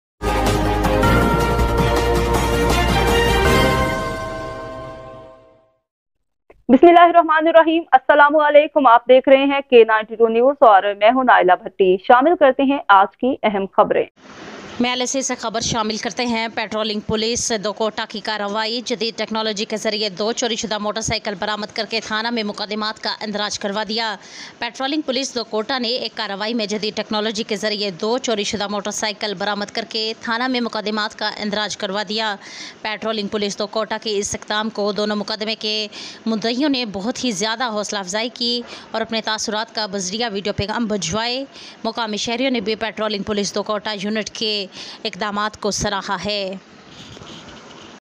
پٹرولنگ پولیس دو کوٹہ کی کارواٸ۔جدید ٹیکنا لوجی کے ذریعے 2 چوری شدہ موٹر سائیکل برآمد کر کے تھانہ
میلسی
پٹرولنگ پولیس دو کوٹہ کی کارواٸ۔جدید ٹیکنا لوجی کے ذریعے 2 چوری شدہ موٹر سائیکل برآمد کر کے تھانہ میں مقدمات کا اندراج کروادیا۔

پٹرولنگ پولیس دو کوٹہ نے ایک کارواٸی میں جدید ٹیکنولوجی کے ذریعے 2 چوری شدہ موٹر سائیکل برآمد کر کے تھانہ میں مقدمات کا اندراج کروادیا۔ پٹرولنگ پولیس دو کوٹہ کے اِس اقدام کو دونوں مقدمے کے مدعیو ں نے بہت زیادہ حوصلہ افزائی کی اور اپنے تا ثرات بھی بزریعہ ویڈیو پیغام بھجوائے۔ مقامی شہریوں نے بھی پٹرولنگ پولیس دوکوٹہ یونٹ کے اقدامات کو سراہا ہے۔